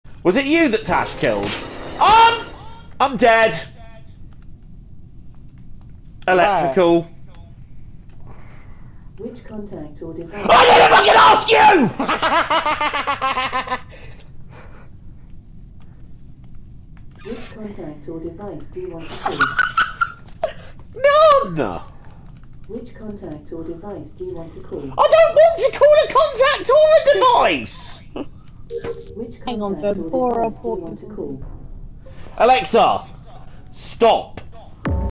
WHY IS ALEXA LIKE THIS
Seriously. I didn't even say anything to summon her!